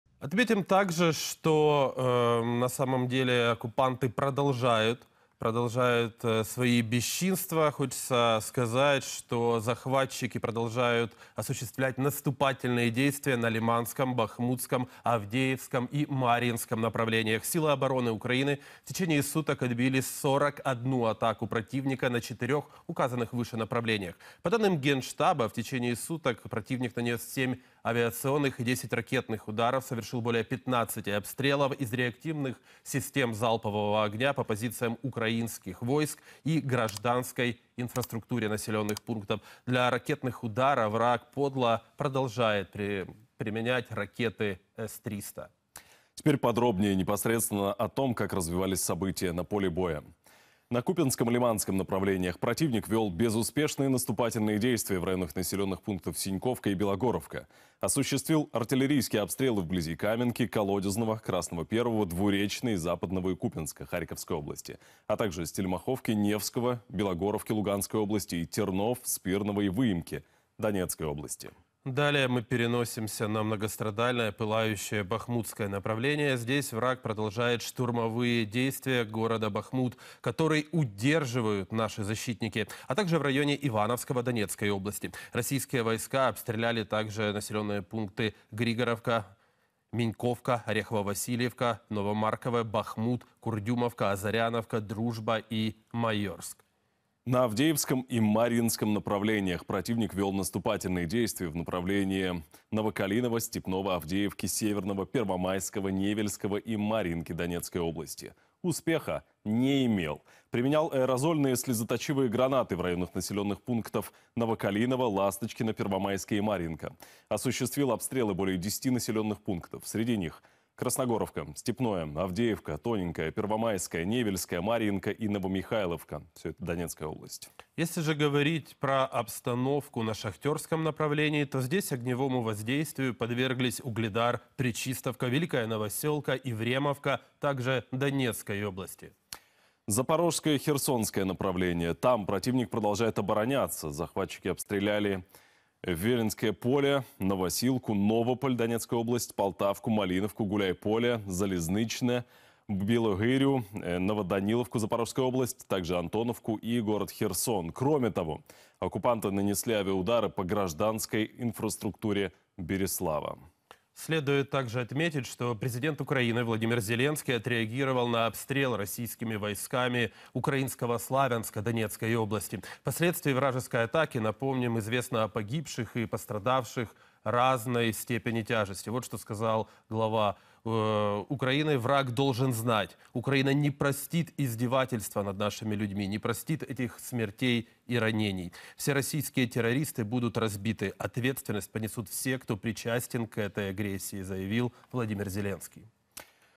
398 сутки войны: карта боевых действий
Оперативная информация от Генерального штаба Вооруженных Сил Украины.